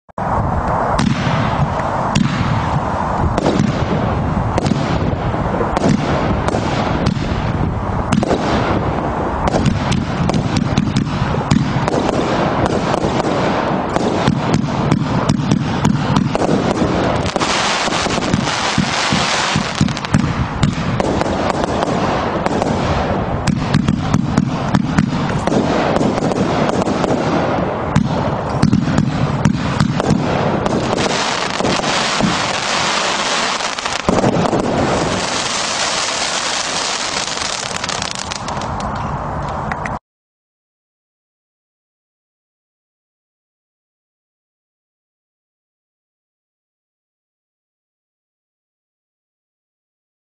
Absolute Fireworks Essential - 49 Shot Barrage @
A fantastic 49 Shot, fast paced cake with wonderful, vivid colours. The Essential 49 Shot battery is spectacular, with effects including Blue Pistil with Red Peony and Red Tails, Purple Pistil with Green Peony and Green Tails, Silver Peony to Silver Blinking with Rain, Gold Coconut with Glitter. A true connoisseur's firework.